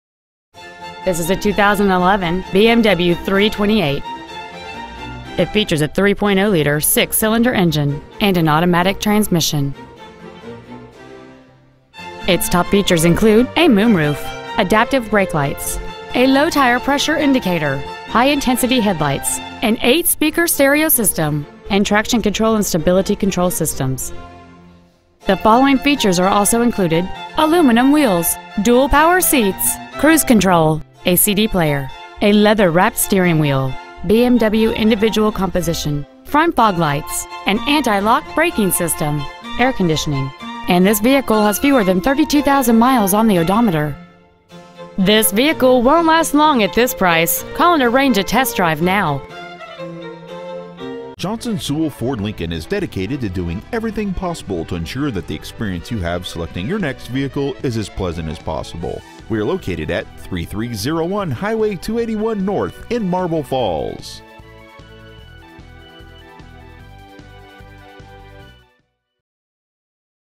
Pre-Owned 2011 BMW 328I Marble Falls TX
Year : 2011
 Make : BMW
 Model : 328I
 Engine : 3.0L I6 24V MPFI DOHC
 Trans . : 6-SPEED AUTOMATIC
 Exterior : BLUE
 Miles : 31,566
 Interior : TAN
 Stock : PD1121